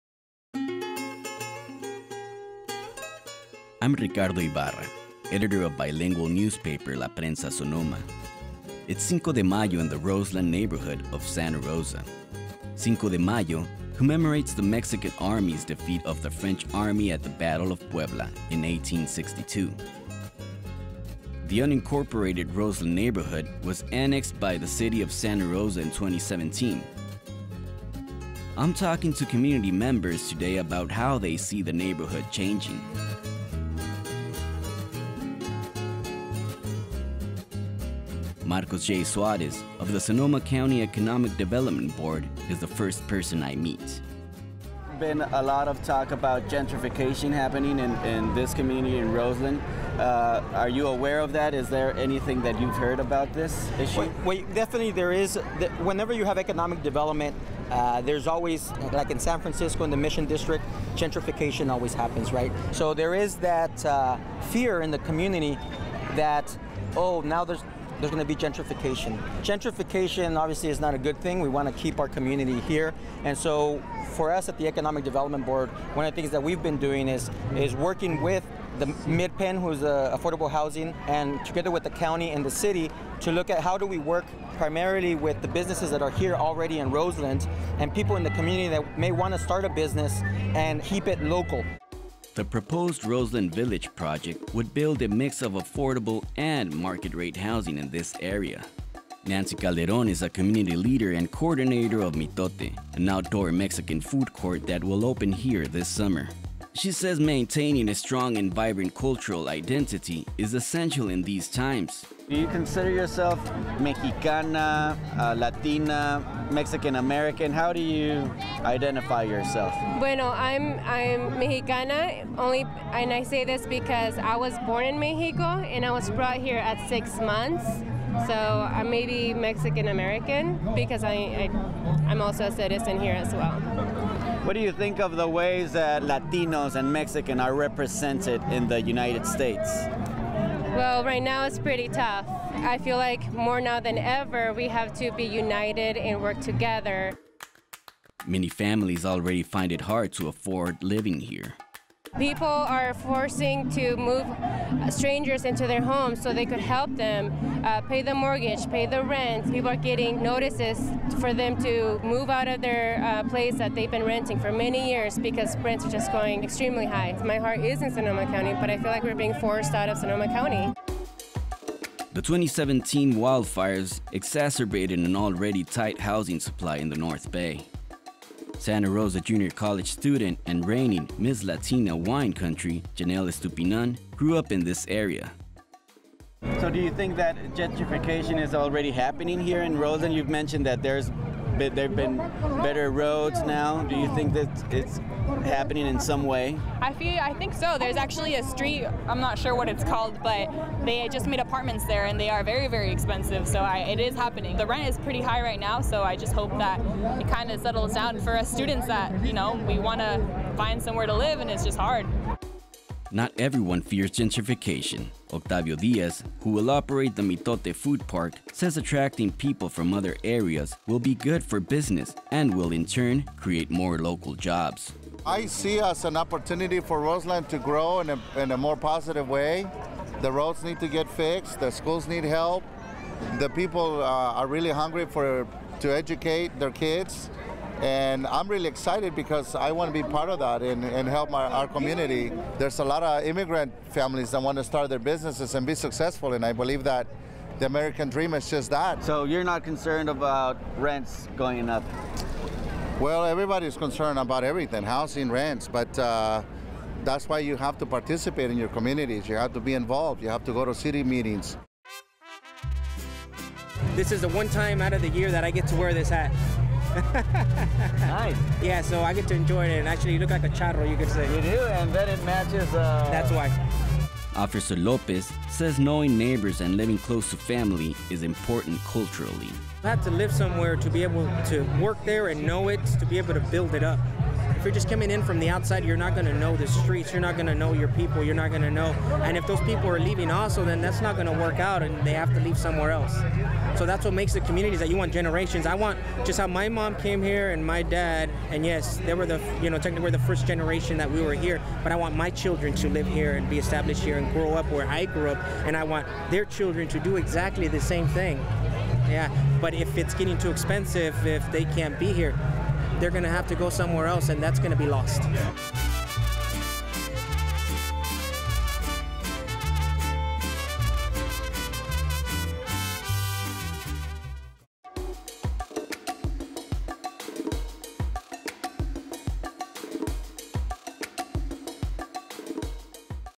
Mi Tierra
Are you worried about getting priced out of Sonoma County? During this year’s Cinco de Mayo festivities, residents in Santa Rosa’s Roseland expressed their hopes and fears for the neighborhood to La Prensa Sonoma editor Ricardo Ibarra. (Producer-Director: Joshua Dylan Mellars)